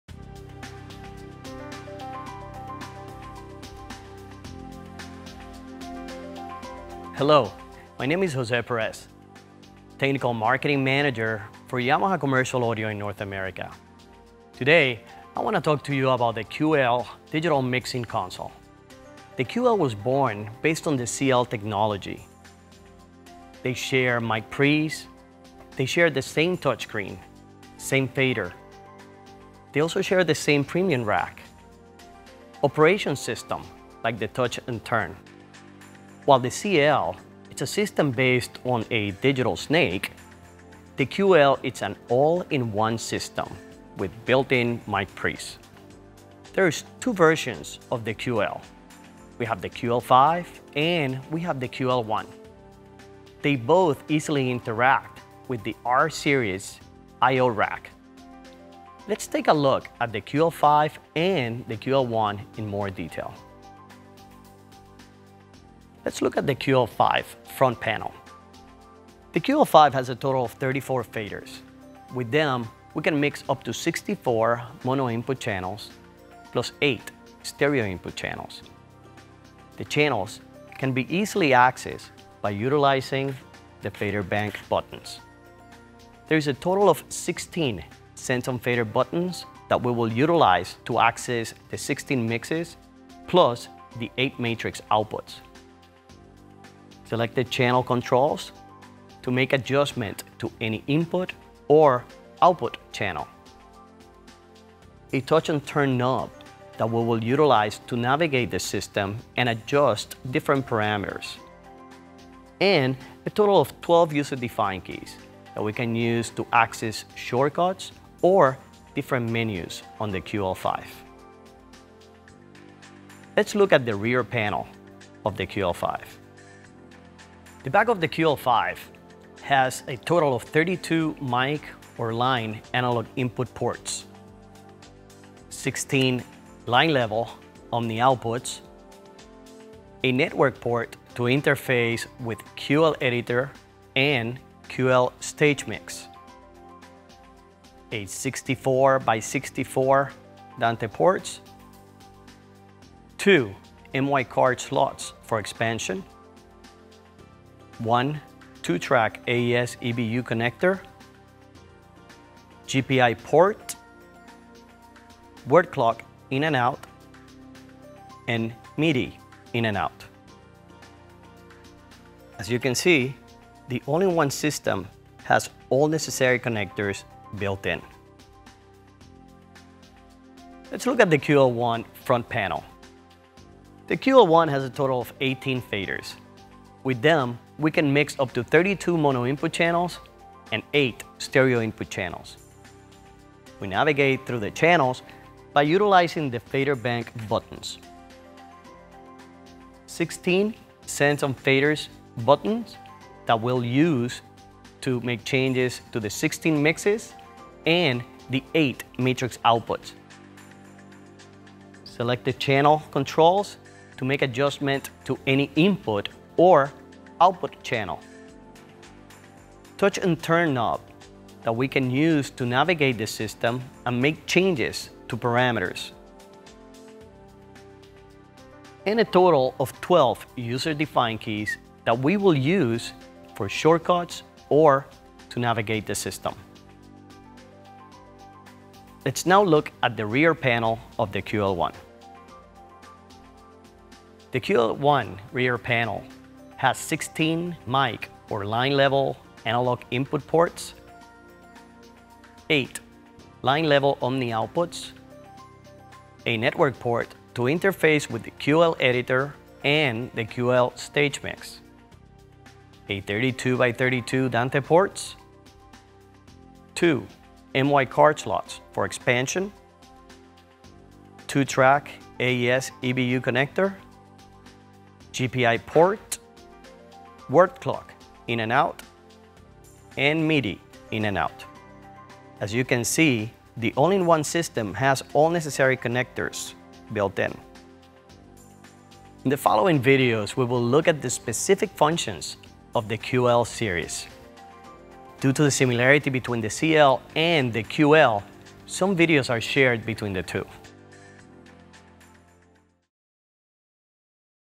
CL/QL Series Training Video: 1.1 QL Console Overview
Experience our CL series and QL series training seminar online!
The features and functions are explained by Leland Green and Jose Perez - top two of the most experienced trainers of Yamaha digital mixing consoles. You can follow the contents in order, or choose a paticular video focusing on a topic.more detail...